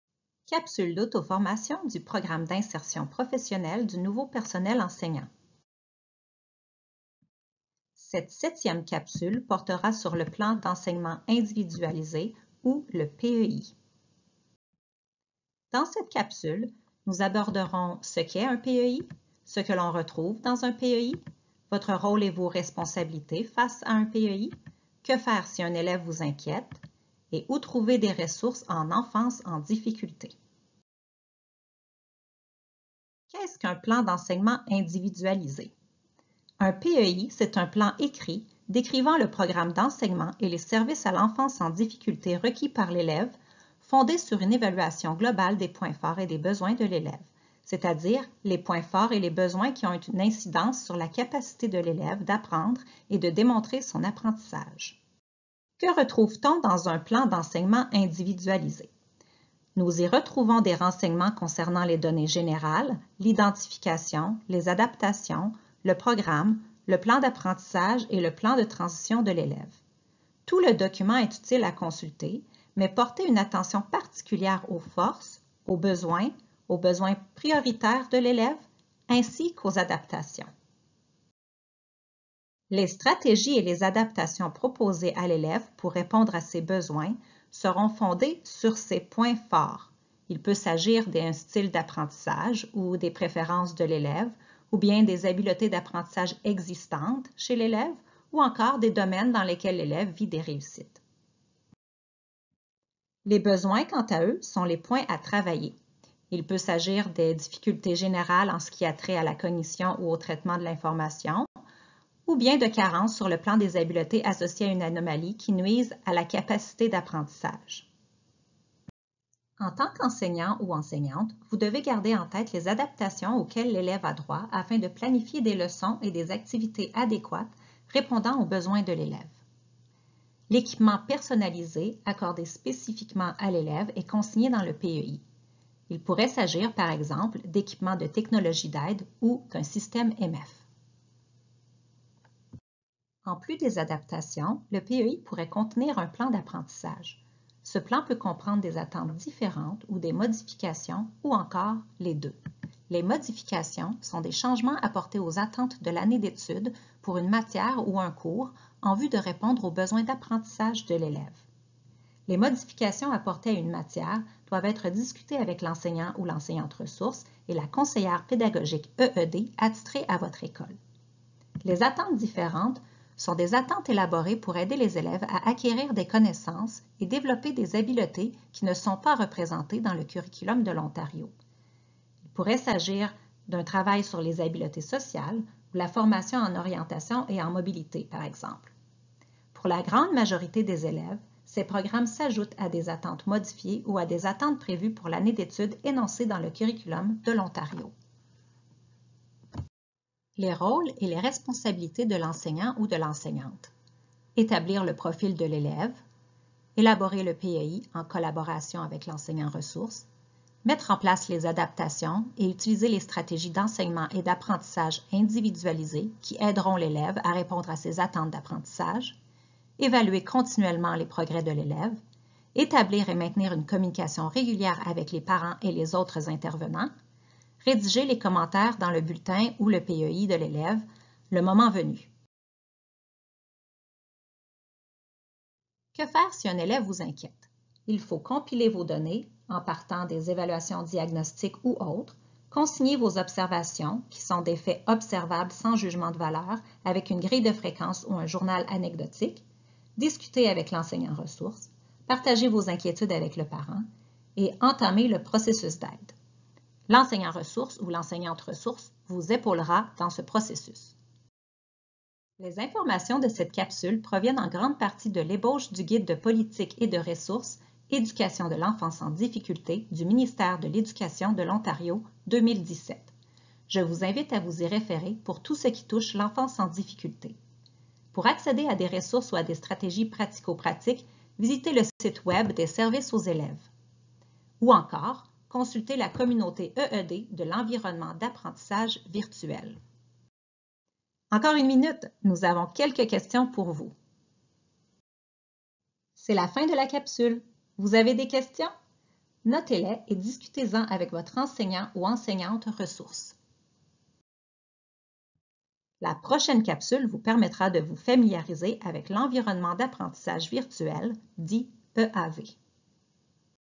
Capsule 7-Plan d'enseignement individualisé (PEI)
Dans cette capsule l’enseignant PIPNPE du Conseil scolaire Viamonde prendra connaissance ce qu’est un PEI, le rôle et responsabilités d’un enseignant face à un PEI, ce que l’on retrouve dans un PEI, que faire si un élève semble vous inquiéter er où trouver des ressources en EED dans l’EAV du CSViamonde.